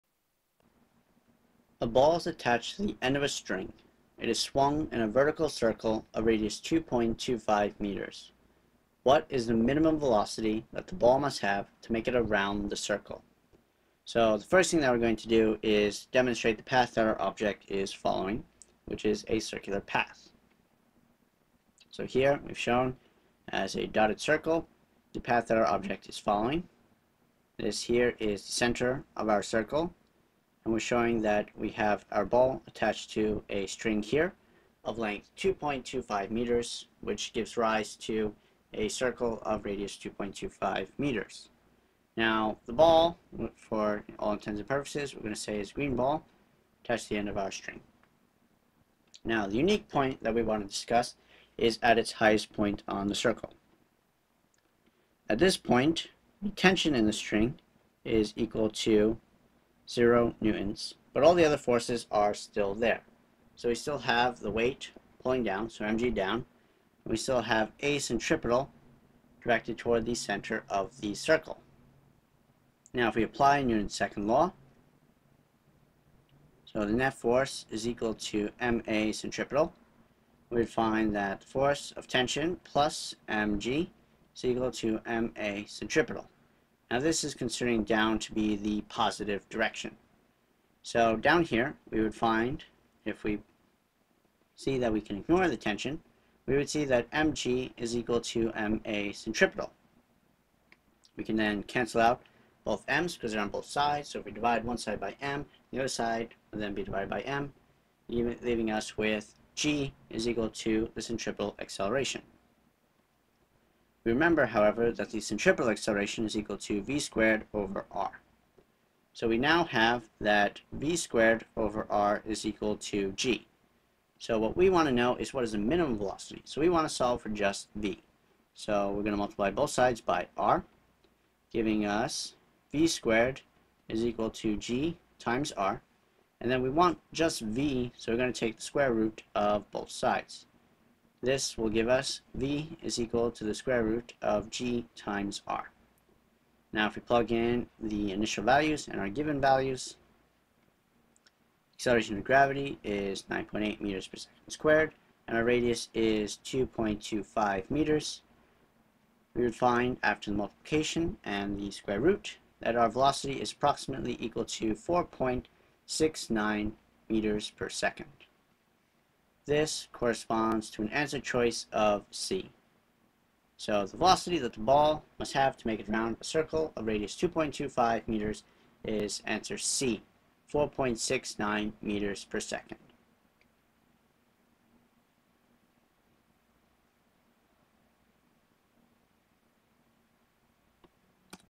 AP Physics B Uniform Circular Motion Presentation #29
A ball is attached to the end of a string. It is swung in a vertical circle of radius 2.25m. What is the minimum velocity that the ball must have to make it around the circle?

More resources including presentations, homework, labs, and assessments for AP Physics B are available on the NJCTL

Slide text:  29 A ball is attached to the end of a string. It is swung in a vertical circle of radius 2.25 m. What is the minimum velocity that the ball must have to make it around the circle? A 3.87 m/s B 4.34 m/s C 4.69 m/s D 5.12 m/s E 5.39 m/s